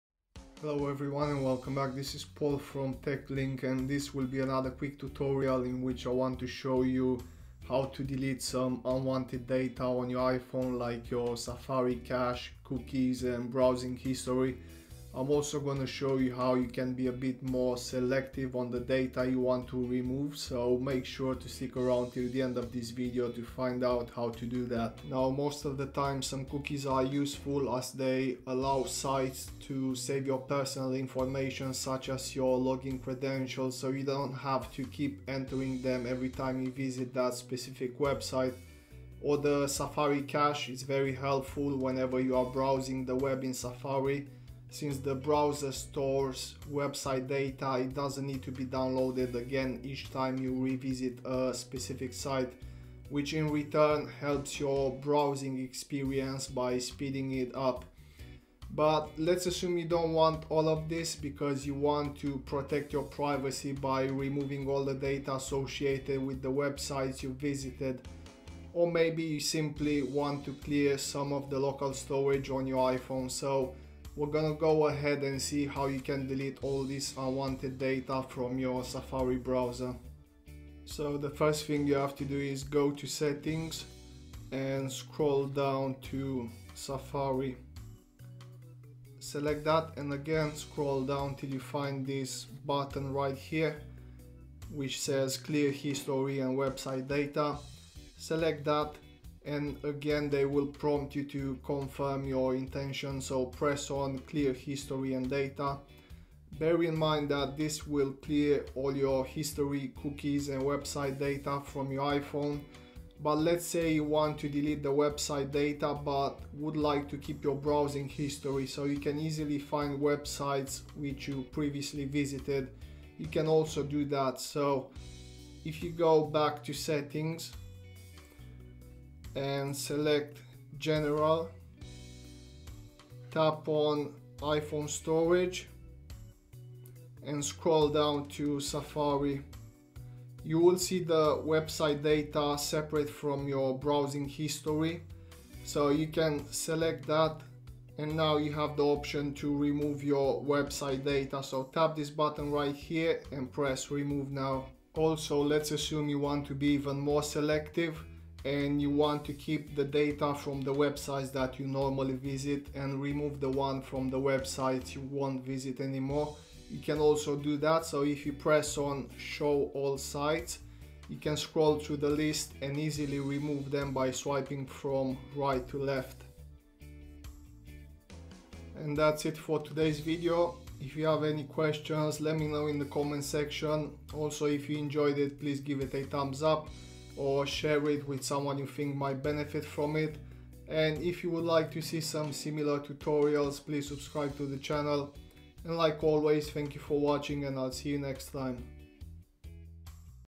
How to Clear your Safari Cache on iPhone and Delete Cookies, Browsing History
This will be another quick tutorial in which I want to show you how to delete some unwanted data on your iPhone, like your Safari cache, cookies and browsing history. I'm also gonna show you how you can be a bit more selective on the data you want to remove.
Most of the times, some cookies are useful, as they allow sites to save your personal information, such as your login credentials, so you don't have to keep entering them every time you visit that specific website. Or the Safari cache, its very helpful whenever you are browsing the web in Safari. Since the browser stores website data, it doesn't need to be downloaded again each time you revisit a specific site, which in return helps your browsing experience by speeding it up.
But lets assume you don't want all of this because you want to protect your privacy by removing all the data associated with the websites you've visited. Or maybe, you simply want to clear some of the local storage on your iPhone. So I'm gonna show you how you can delete all this unwanted data(Cookies, Browsing History, Cache) from your Safari browser on an iPhone.

Time Stamp
0:00 Intro
1:20 How to Delete Cookies, Browsing History and Clear your Safari Cache
1:54 How to delete Cache Files from Safari on iPhone
2:35 How to selectively delete Cache Files from Safari on iPhone
2:59 Outro

You can find here my previous video on
How to Clear Safari Reading List on iPhone | Delete Web Pages from Safari Reading List on iOS 14

Support TechLink on Amazon